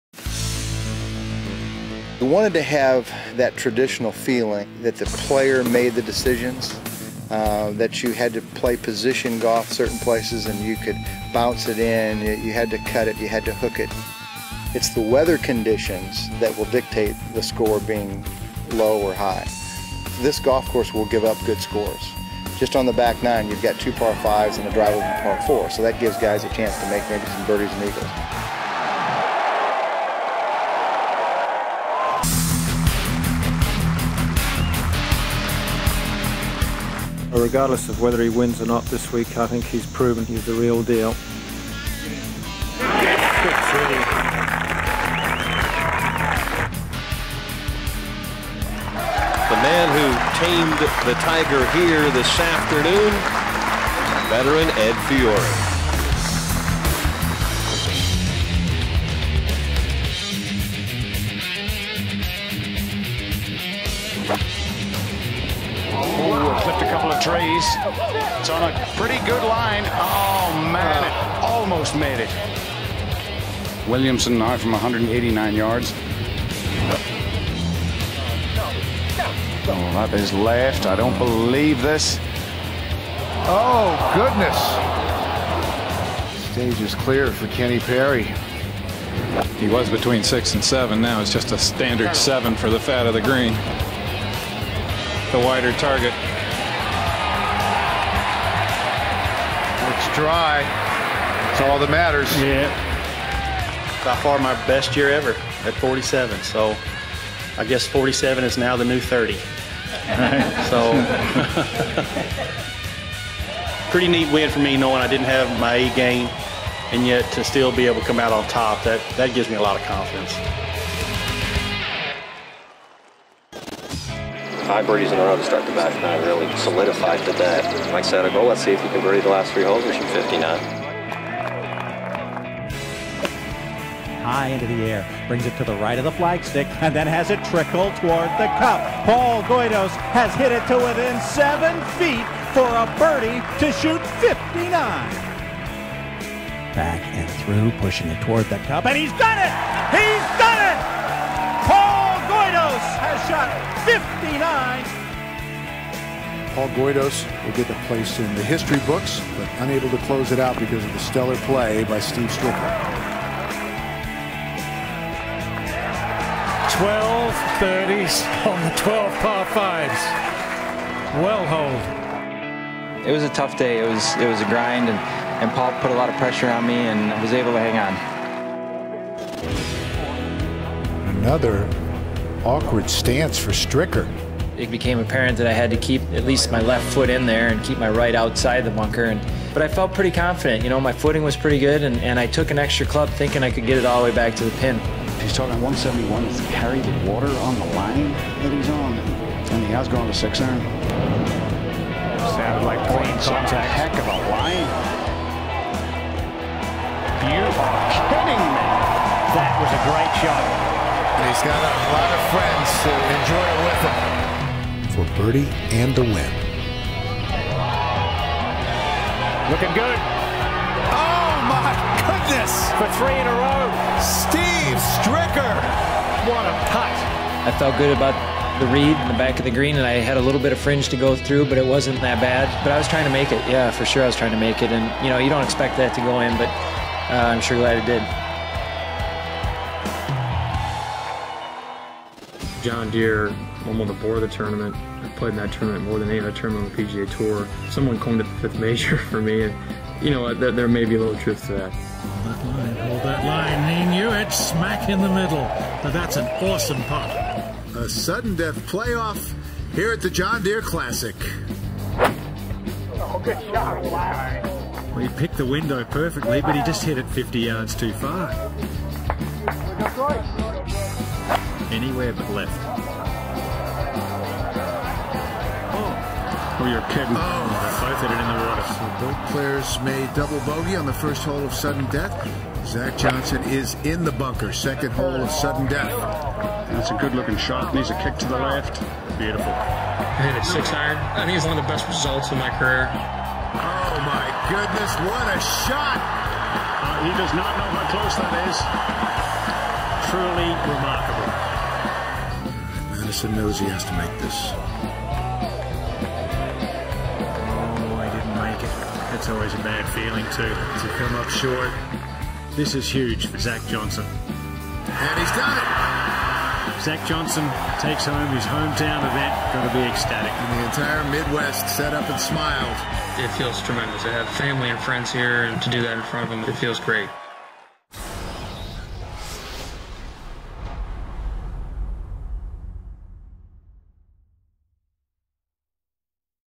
Signature Moments: John Deere Classic
Watch David Frost's wins, Ed Fiori holding off Tiger Woods, Kenny Perry's  2008 victory, Paul Goydos 59, a pair of Steve Stricker wins and Zach Johnsons clutch win in 2012. .